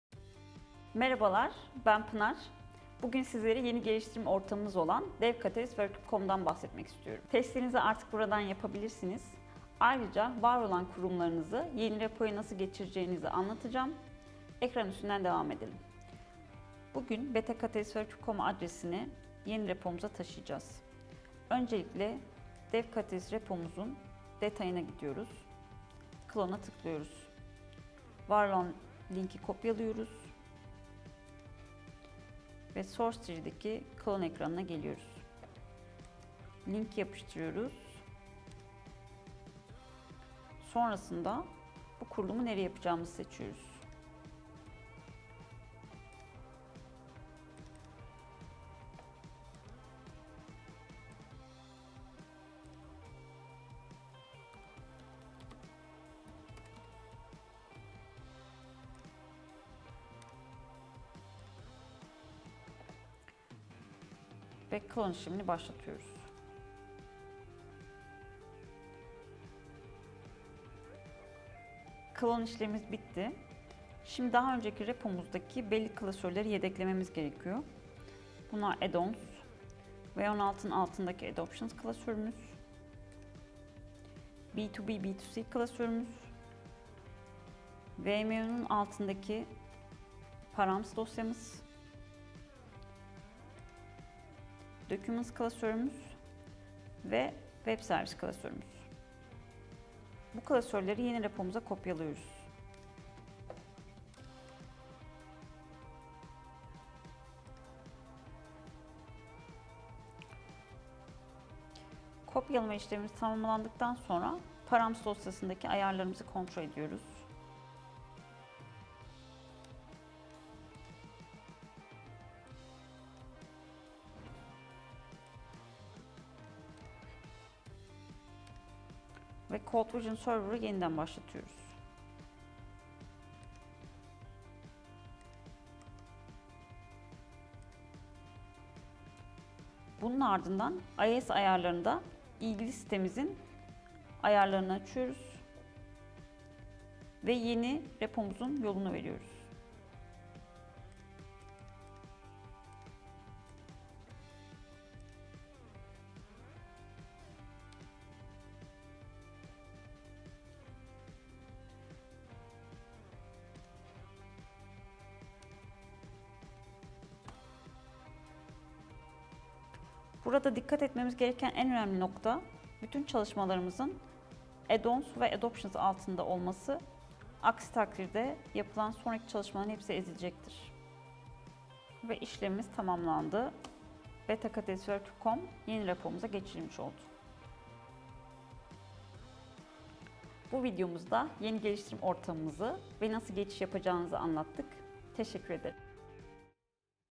Yeni Dev Catalyst Geliştirim Ortamına Geçiş Nasıl Yapılır?
Artık testlerinizi yeni geliştirim ortamımız devcatalyst.workcube.com ortamında yapabilirsiniz.
Devcatalyst ortamı, Workcube Catalyst sisteminin size her zaman son versiyonunu sunar.
Video içerisinde Devcatalyst ortamının nasıl kurulduğunu ve varolan kurulumlarınızı bu ortama nasıl geçireceğiniz ayrıntılı şekilde sunuluyor.
Source tree ekranından clone işleminin ne şekilde yapıldığını, temel IIS ayarlarının nasıl olması gerektiğini ve yedekleme işlemlerini uygulamalı şekilde göreceksiniz.